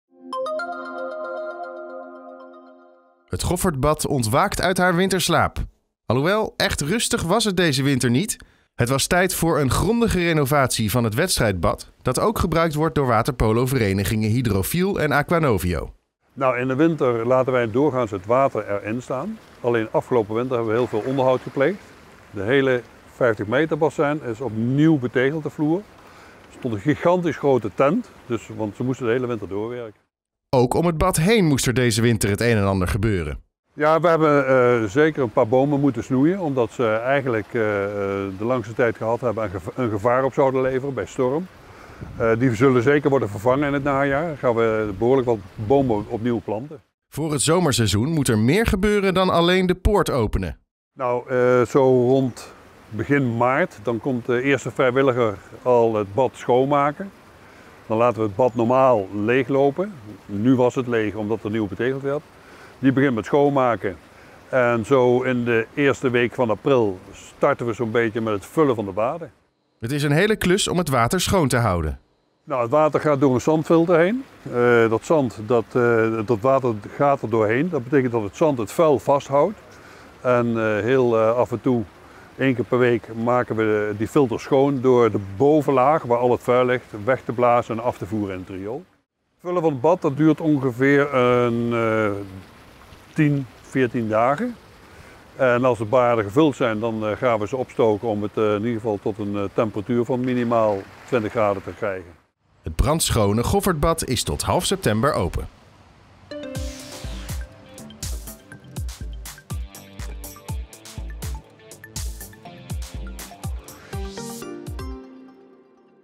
Na winterse renovatie is het Goffertbad weer open
NIJMEGEN - Na een niet al te rustige winterslaap ontwaakt het Goffertbad. De bodemtegels van het wedstrijdbad, dat ook gebruikt wordt door waterpoloverenigingen Hydrofiel en AquaNovio, zijn vervangen. “Er stond deze winter een gigantisch grote tent, hieronder werd hard gewerkt om het zwembad weer klaar te stomen voor het zwemseizoen,” vertelt directeur Heino Jacobs.